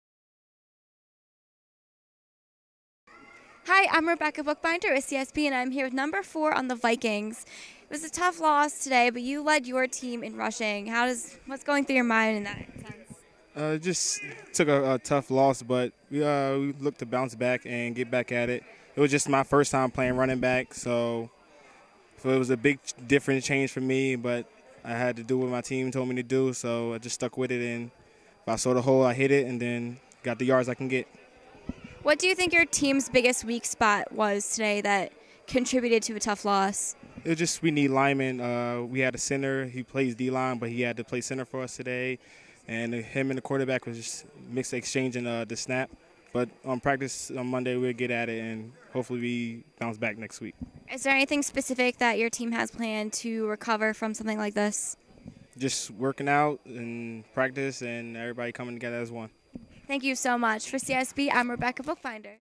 04022016 Vikings Rashon Cooper Interview
IGL Player Jerry Davis  for The VIKINGS, was Interviewed by CSB's - IGL Weekly On-Field Reporter Rebecca Bookbinder, after The Season Opening Day's Game